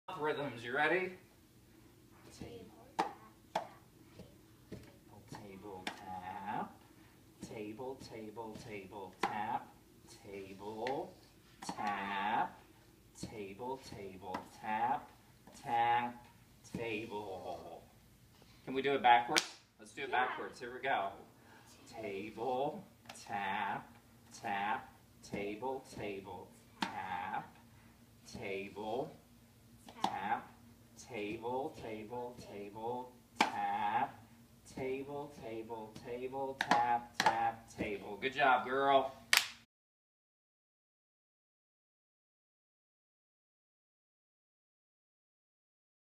Andrews's student idea for creating cup tapping pumpkin rhythms
Andrew Young encourages this young piano student to be creative in how she practices her pumpkin rhythm cards. The student gets creative, Andrew gets creative, and the student has a wonderful time learning and internalizing rhythm!